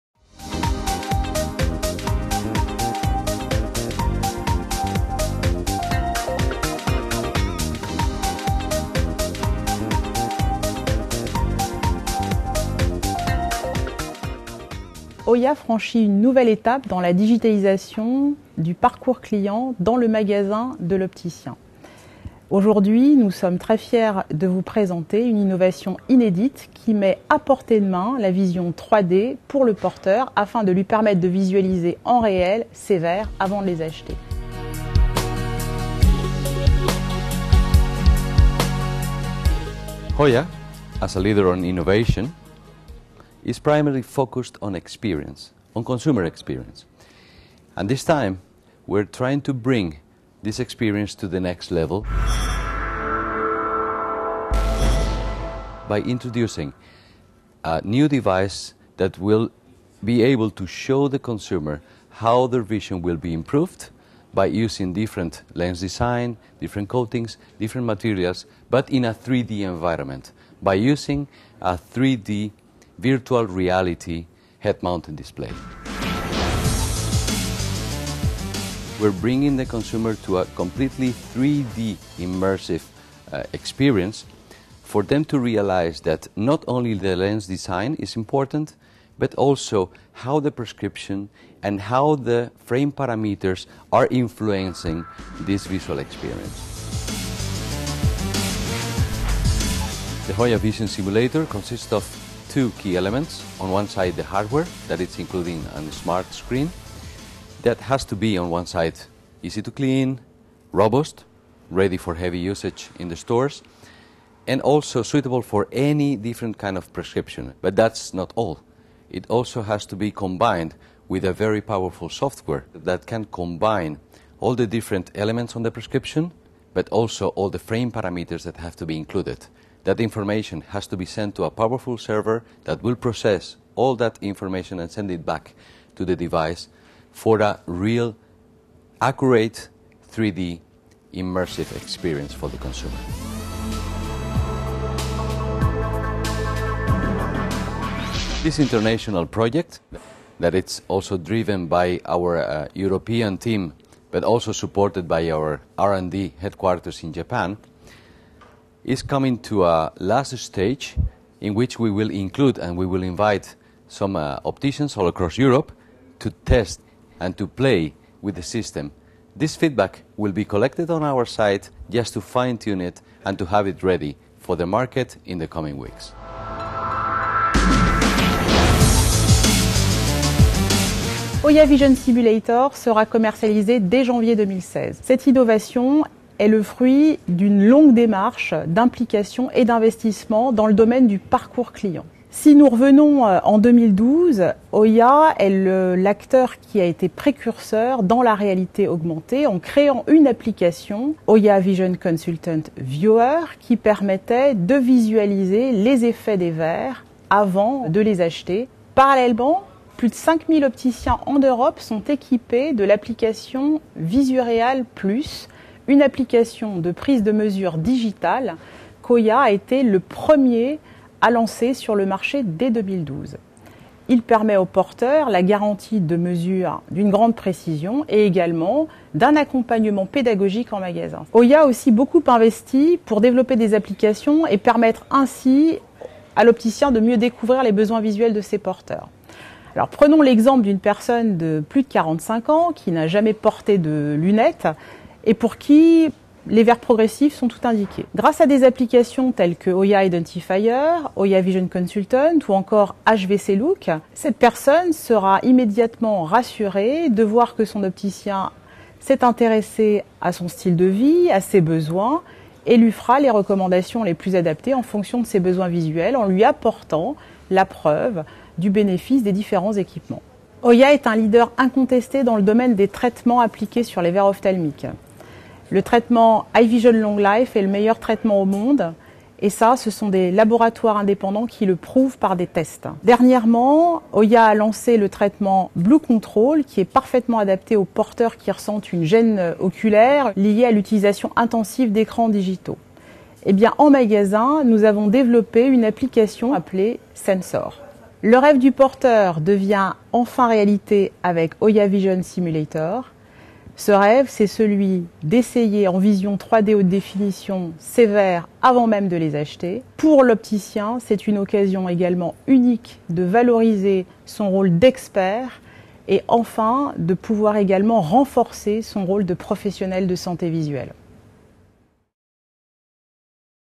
[Reportage] Hoya Vision Simulator
Les brochures et autres démonstrations sur écran sont considérées comme fastidieuses et difficilement assimilables par certains porteurs. C’est pourquoi, Hoya commercialise à compter de janvier 2016 Hoya Vision Simulator, un casque qui offre la possibilité aux consommateurs d’essayer en vision 3D leurs nouveaux verres avant de les acheter.